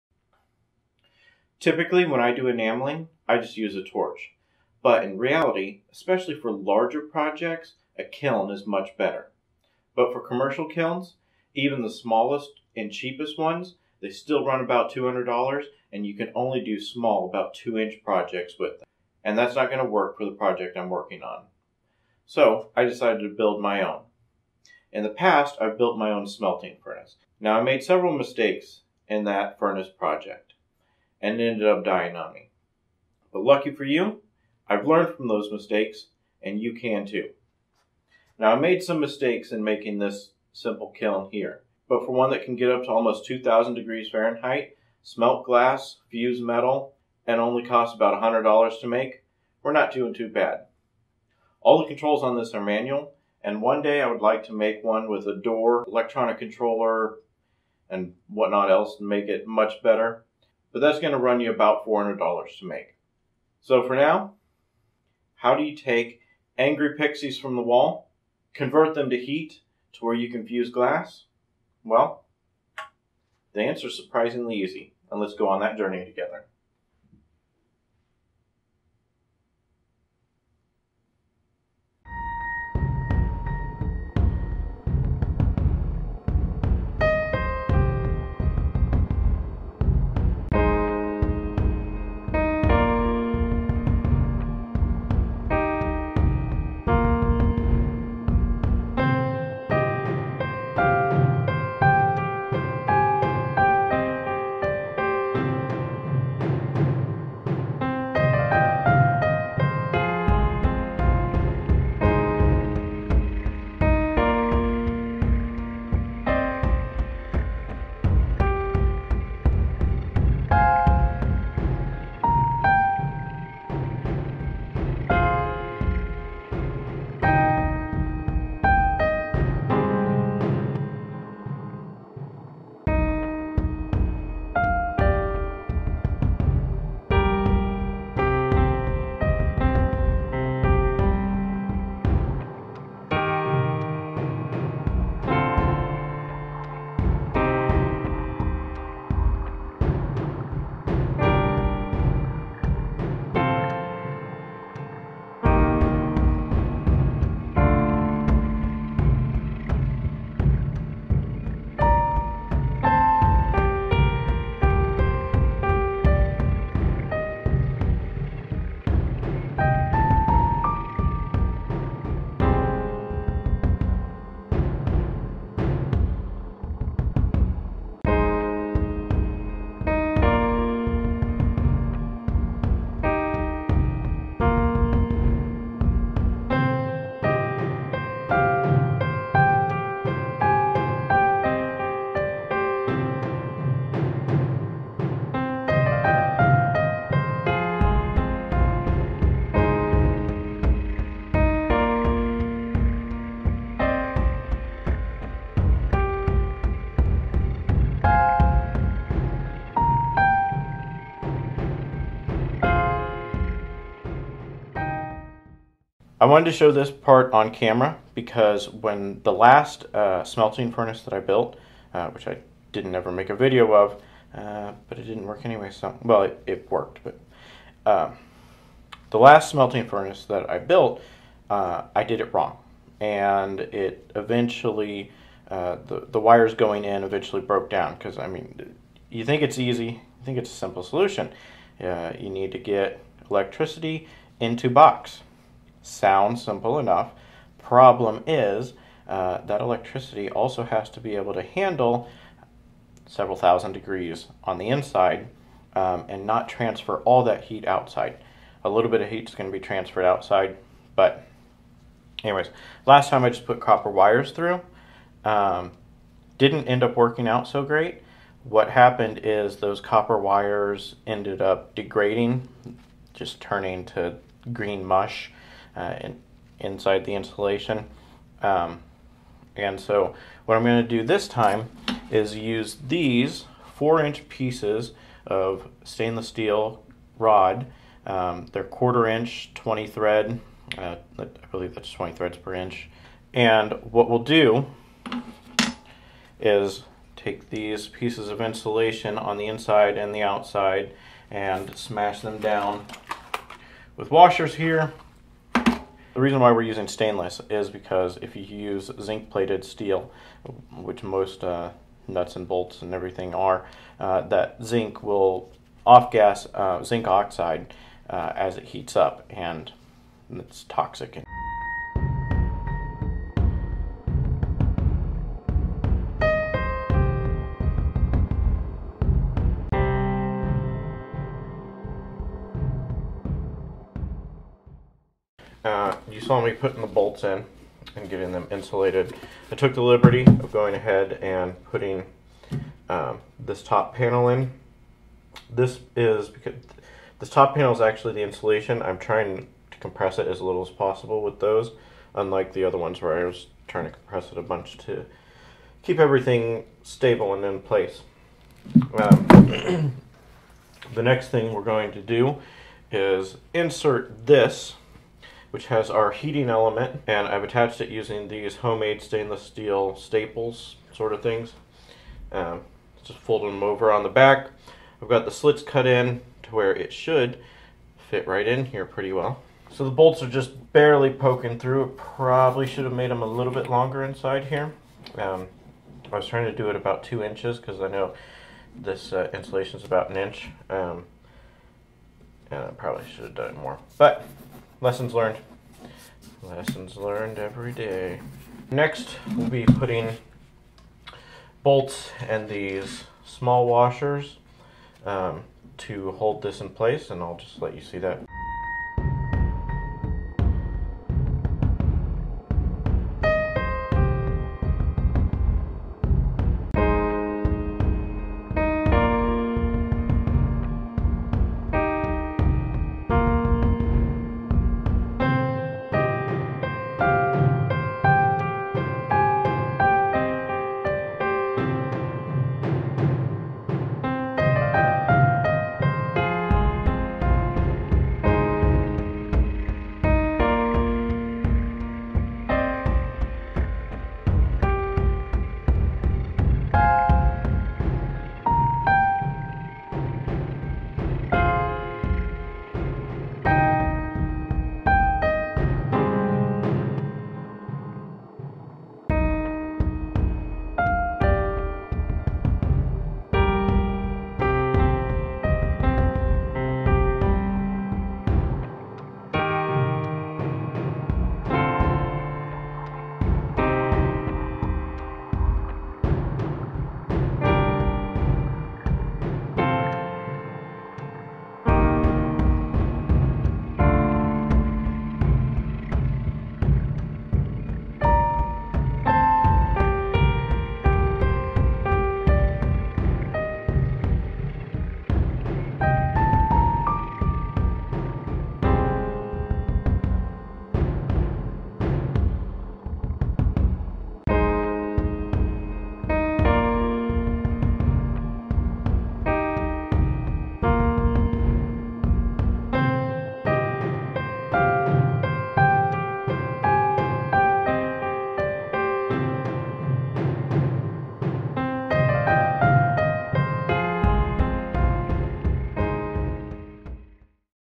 DIY Simple Enamel Glass Kiln for under $100
In this video I make a kiln capable of being used for glass enameling.  Actually the technical limits of this kiln should be around 2,500 degrees Fahrenheit, so it should be able to do a lot more than just fuse glass.  Fuse metal for metal clay projects.  It doesn't have a temperature controller, so I would only use it for silver clay.  Copper and Bronzclay precious metal clays probably won't work out so well because they need to be fired on a schedule.

In this I reference a one day build from the Adam Savage's Tested channel, and how I wished I had done my heating element coils using the same configuration.